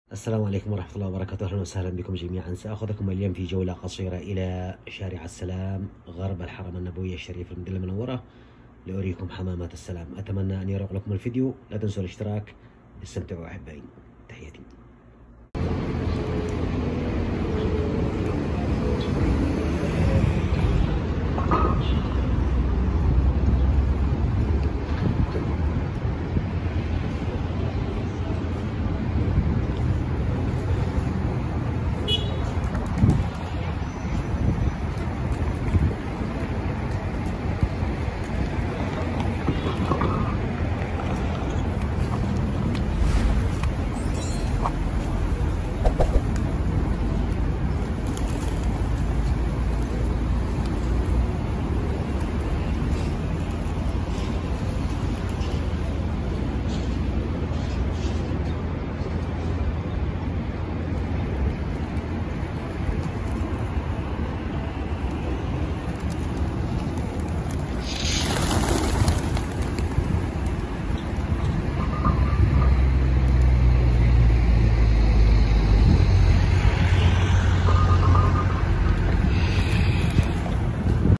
طيور حمام شارع السلام غرب المسجد النبوي في المدينة المنورة
خرجت من غرب المسجد النبوي الى شارع السلام تماما اسفل الكباري وشاهدت حمامات السلام الجميلة التي تبعث البهجة والسرور في القلب والروح شاهد معي طيور الحمام في شارع السلام في المدينة المنورة

سافر_بين_رموش_الطبيعة

جوامع صورتها اليوم في القصيم | جامع الامام محمد بن عبد الوهاب |النخيل في الشماسية | الريان الخبراء